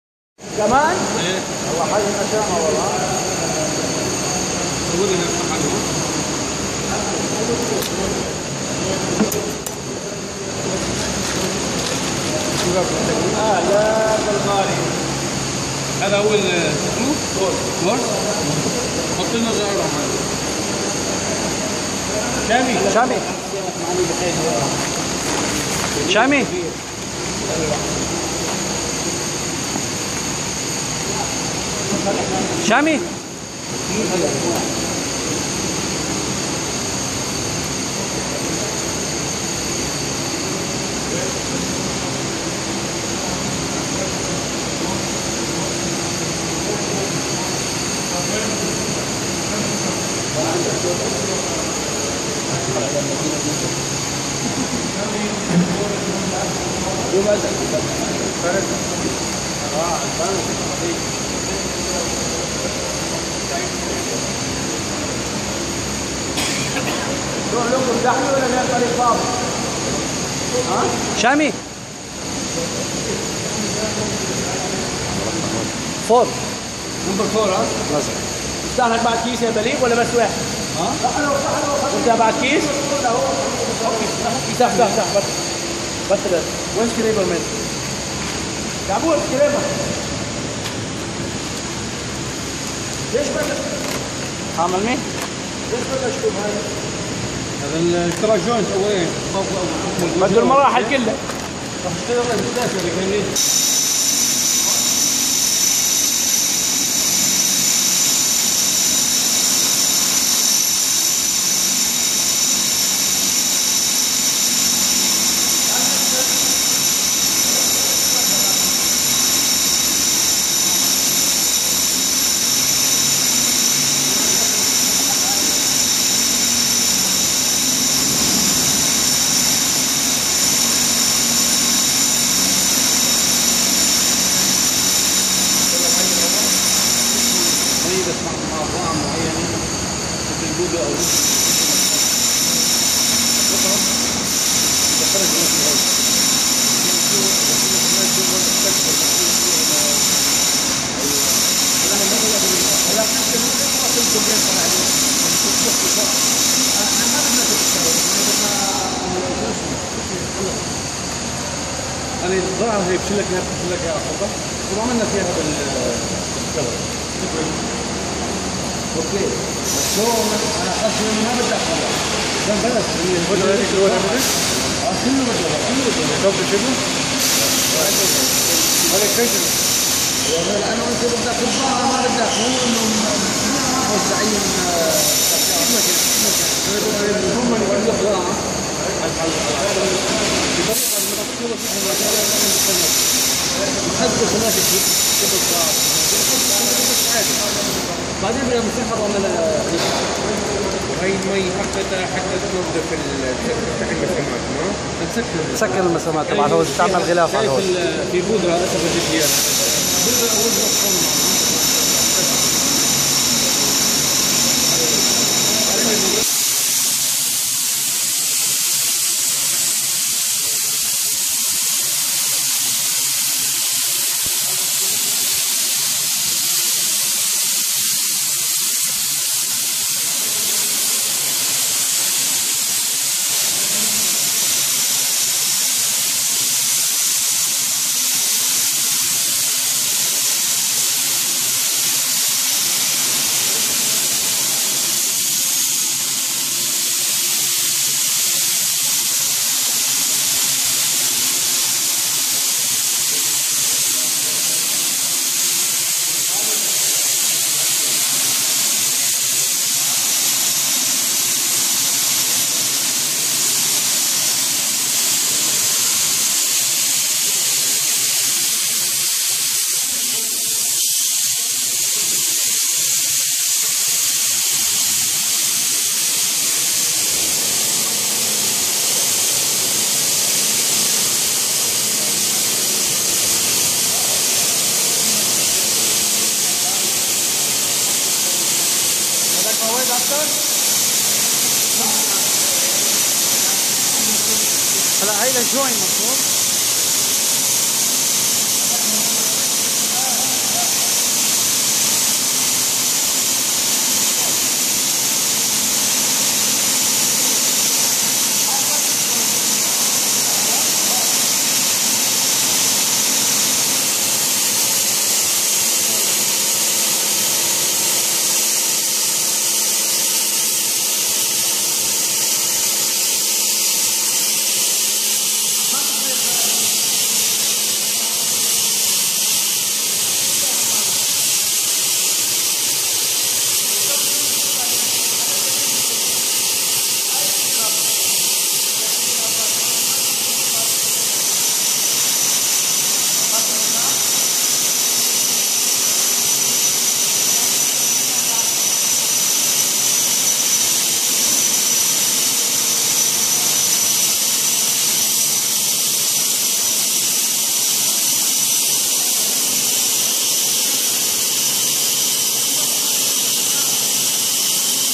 Wagner PC 830 Spraying Plaster Coat of National Paints
Spraying 1700 Sqm of plaster in one day